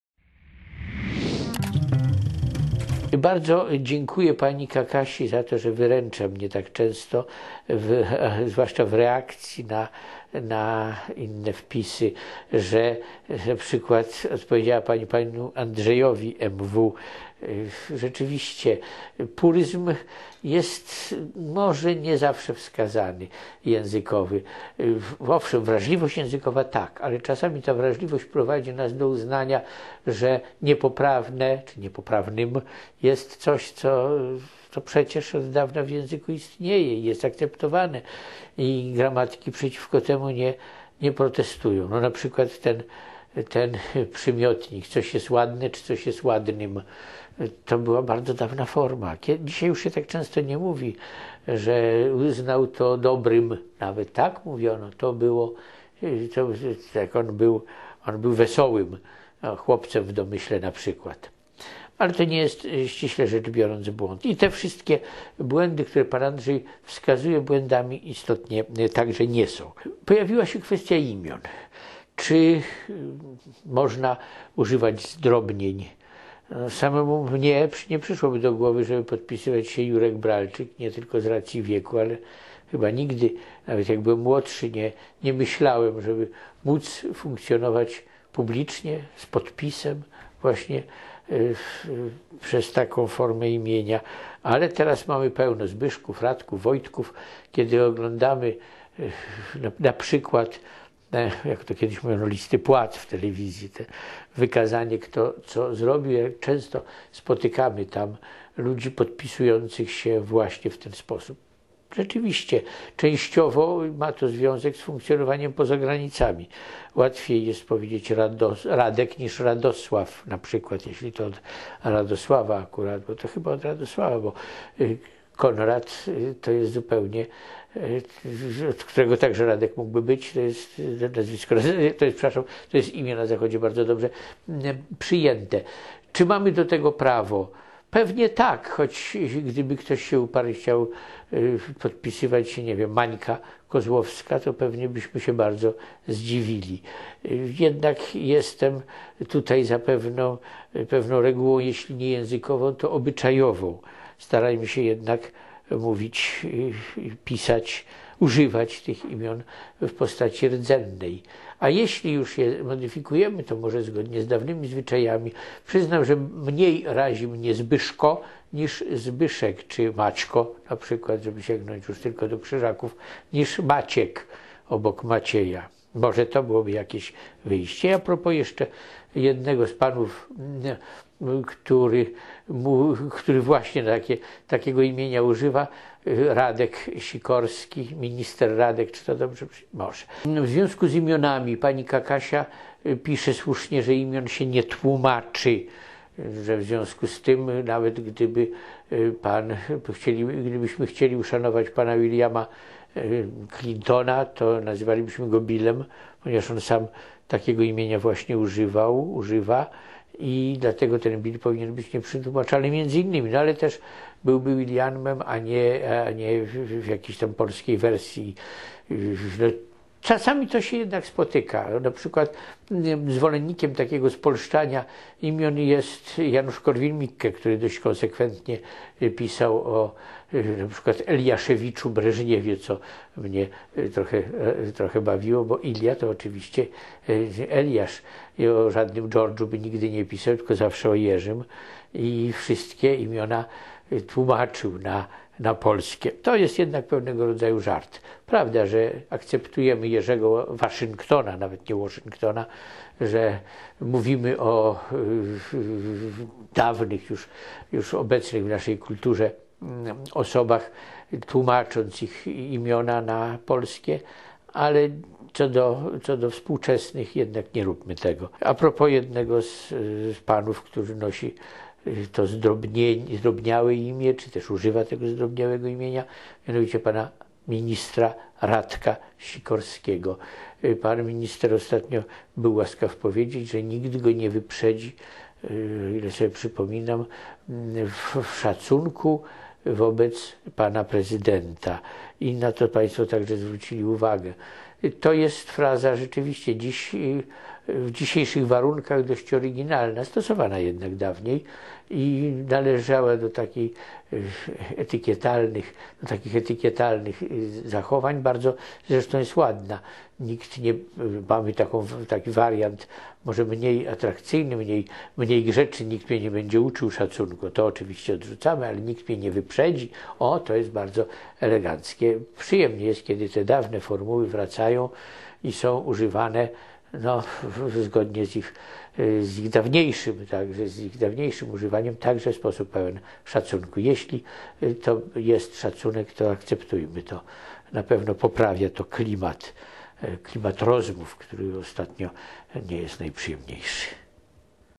Jerzy Bralczyk: Radek czy Radosław Sikorski?
Prof. Bralczyk pyta m.in.czy i jak modyfikować imiona.